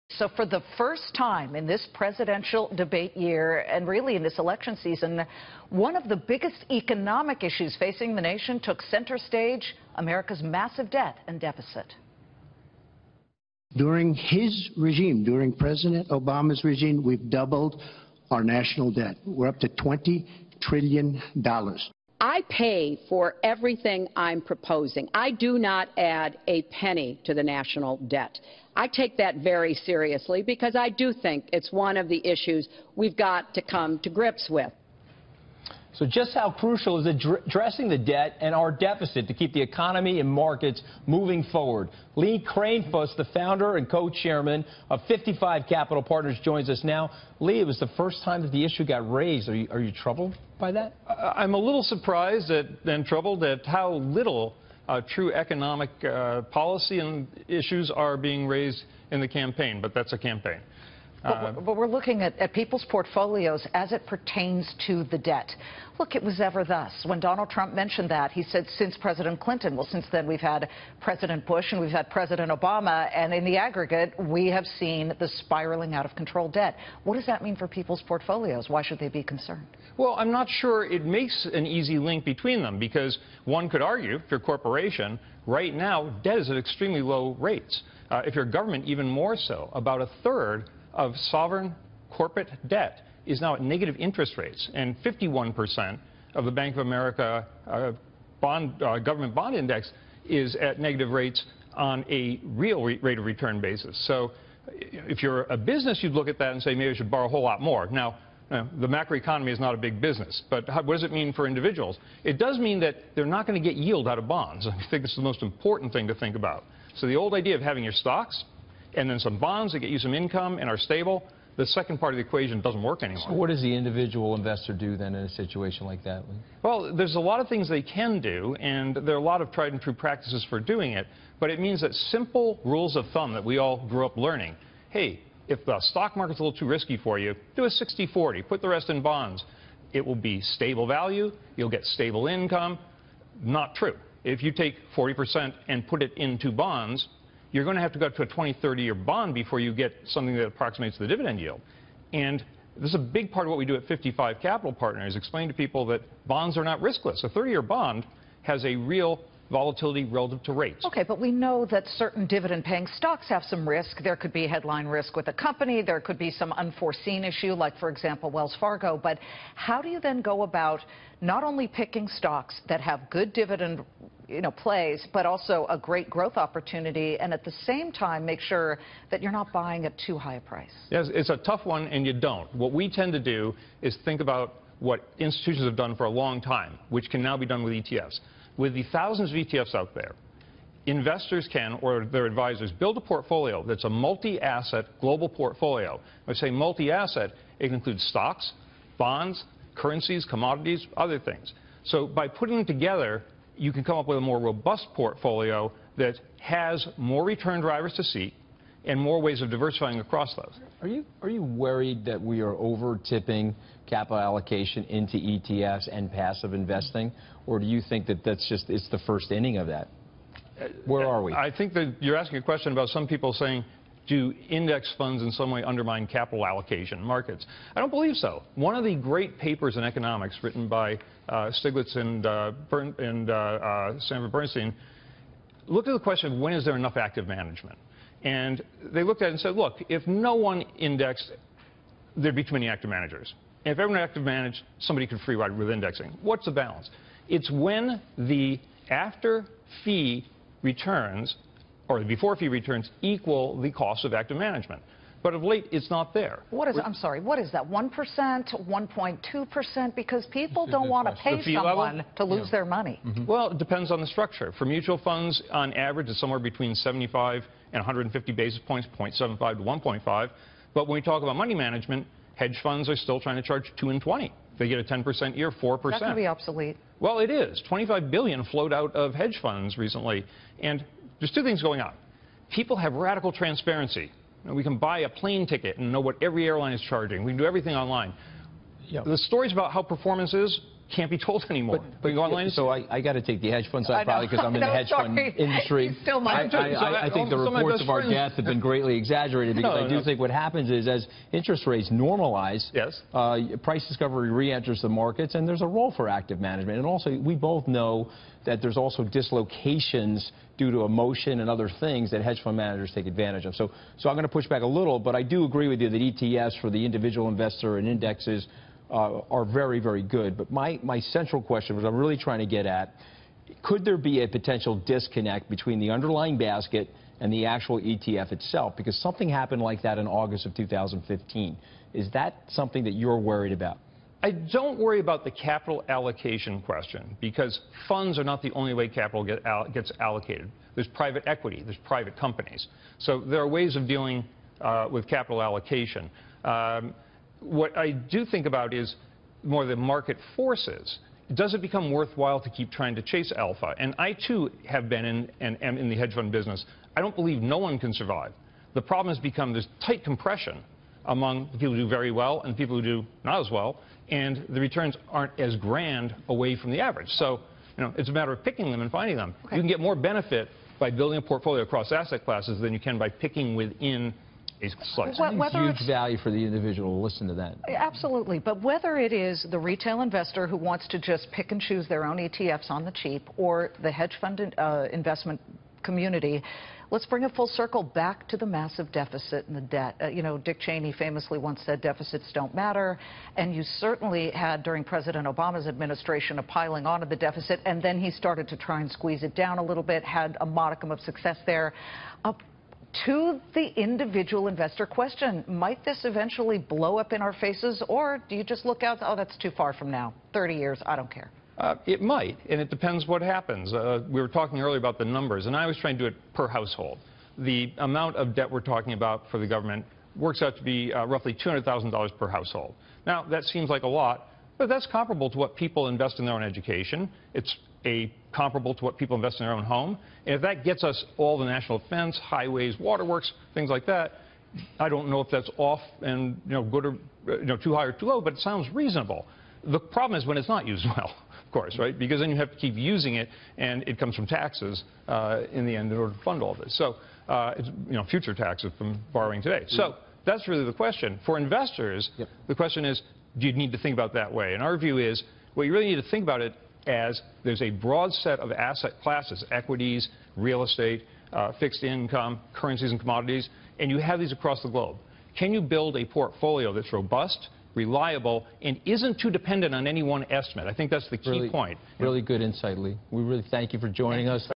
ETFs the key to building a robust portfolio?
Lee Kranefuss of 55 Capital Partners on the economy, investing in ETFs and the government deficit.